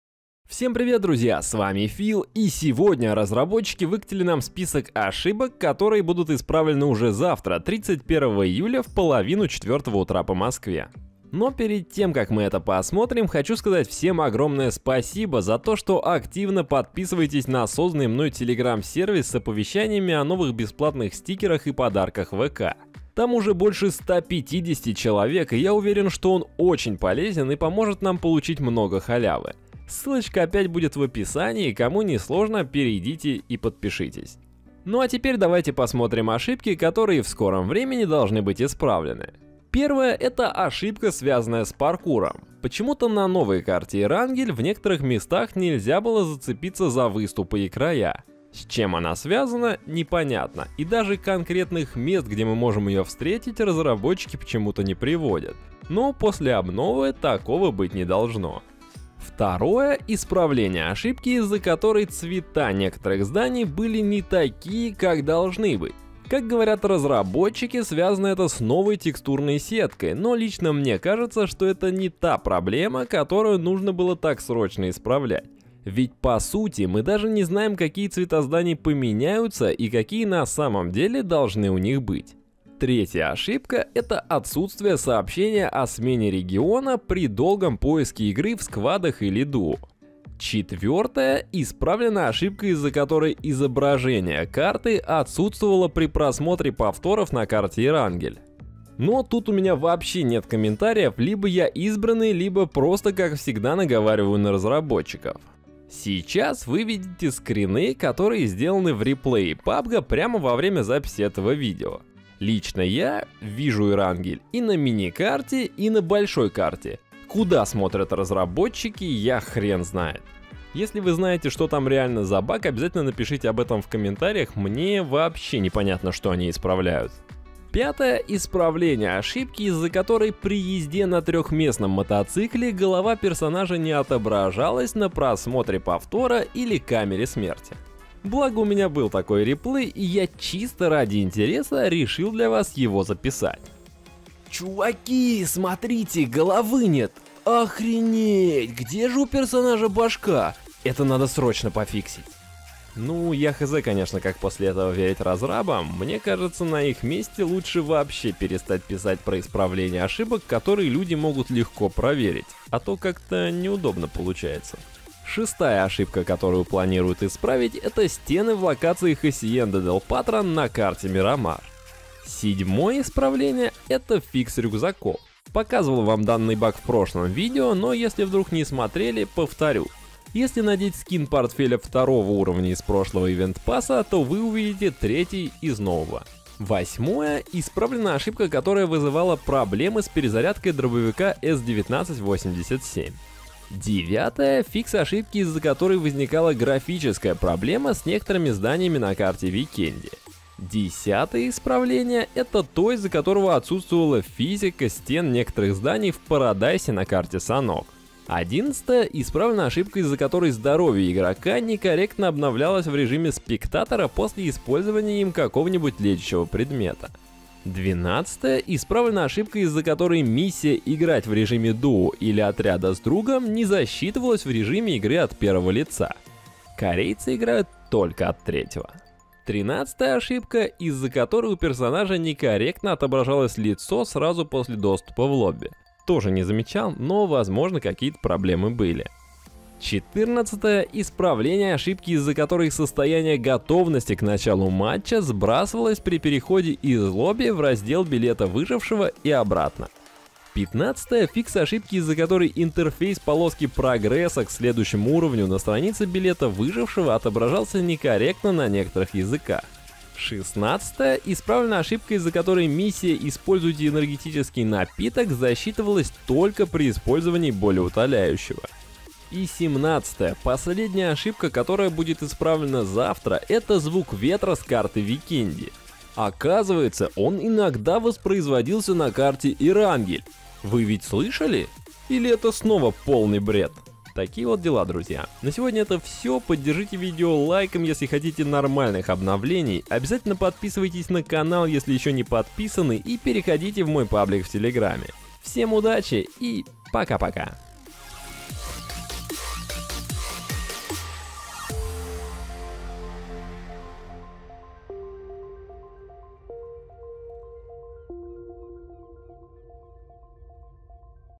PUBG САМЫЕ ВАЖНЫЕ ИСПРАВЛЕНИЯ В НОВОМ ОБНВЛЕНИИ ПАБГ / PLAYERUNKNOWN'S BATTLEGROUNDS НОВОСТИ
PUBG ОБНОВЛЕНИЕ - САМЫЕ ВАЖНЫЕ ИСПРАВЛЕНИЯ

Данное обновление пабга выйдет на основные сервера уже завтра 31 июля, оно будет сопровождаться техническими работами и направлено на исправление ошибок, большинство из которых вы никогда не встречали.
Русский перевод обновления (новостей) по игре PlayerUnknown’s Battlegrounds. Полный обзор, все о чем говорят разработчики.
Новости PUBG от 30 июля 2019 года.